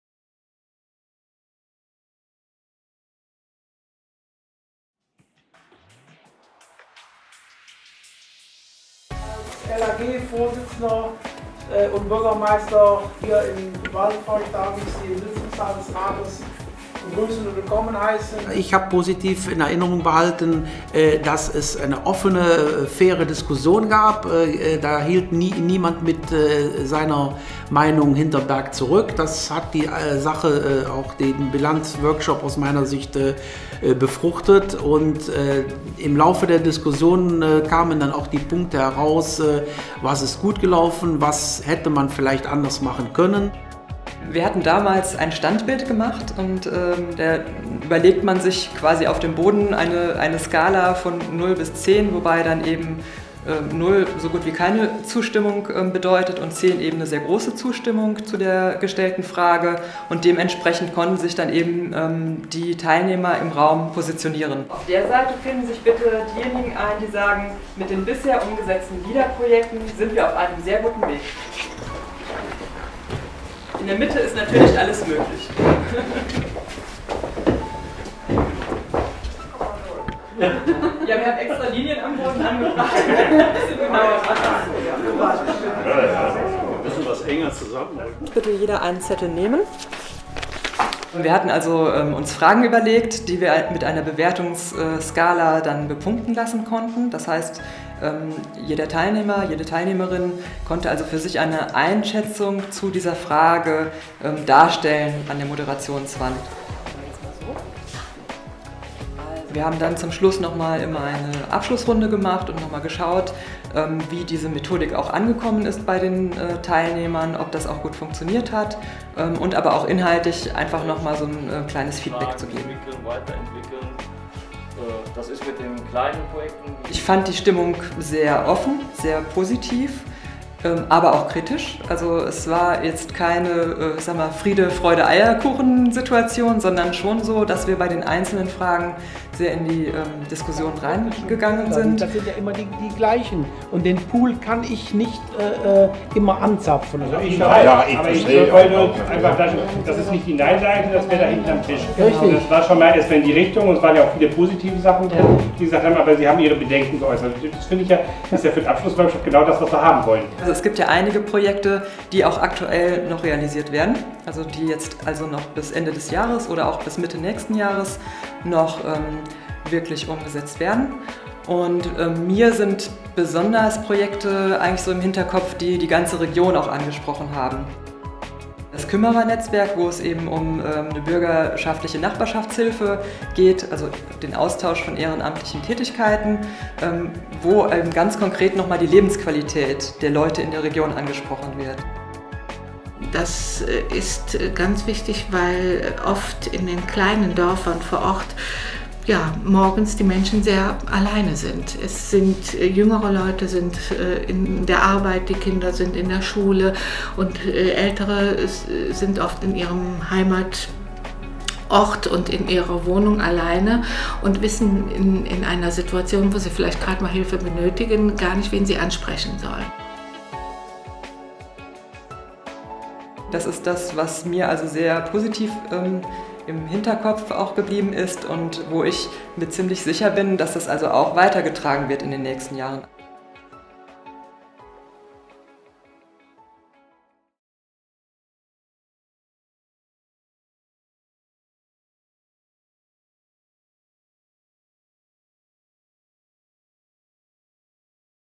Der Selfkant
In der Region „Der Selfkant“ geht es um die Bilanz der Regionalentwicklung mit LEADER. Der Videoclip gibt eine lebendige Diskussion unter Akteuren der Aktionsgruppe wieder und einen Einblick in verschiedene Methoden, mit denen eine Bilanzveranstaltung gestaltet werden kann.

Die Veranstaltungen und der Regionalentwicklungsprozess werden von der Regionalmanagerin und dem Bürgermeister der Gemeinde Waldfeucht kommentiert. Zudem wird mit dem Kümmerer-Netzwerk noch ein regionales Projekt vorgestellt.